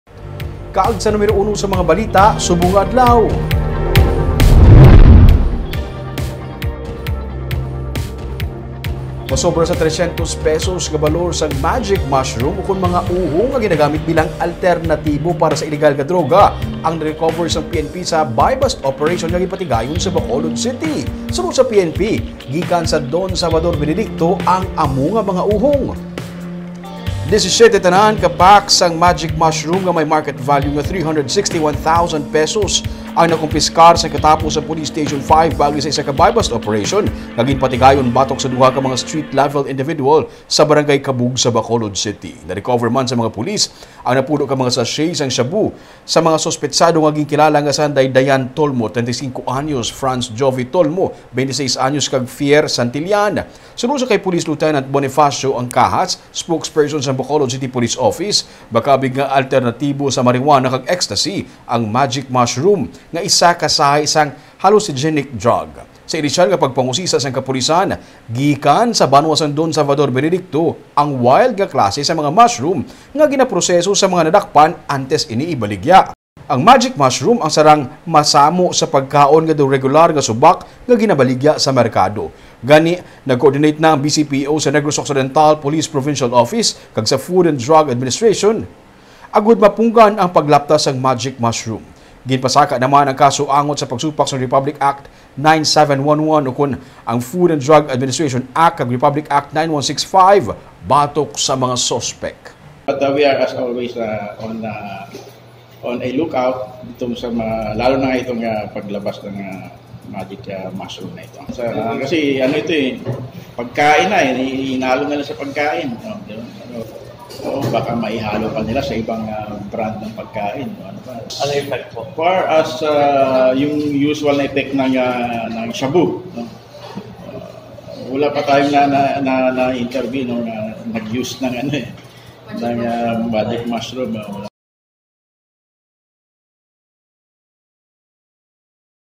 Magic mushroom
Magic mushroom ukon uhong nga ginahimo alternatibo sa iligal nga droga na recover sang PNP sa buy-bust operation